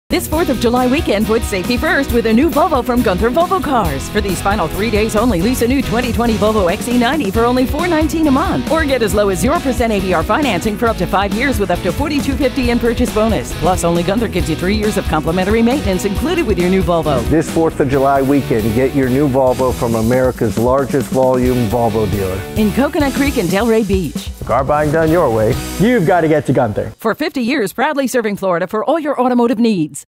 Gunther Volvo Cars XC90 4th Of July Commercial 2020
This Fourth of July weekend

Put safety first
With a new Volvo from Gunther Volvo Cars.
For a limited time,
lease a new 2020 Volvo XC90 for only $419 a month.
Or get as low as 0% APR financing for up to 5 years.With up to $4,250 in Purchase bonus
Plus only Gunther gives you 3 years of complimentary maintenance included with your new Volvo.
Get your new Volvo at America’s Largest Volume Volvo Dealer.
In Coconut Creek and Delray Beach
For car buying done your way,
You gotta get to Gunther
For 50 years proudly serving Florida for all your automotive needs.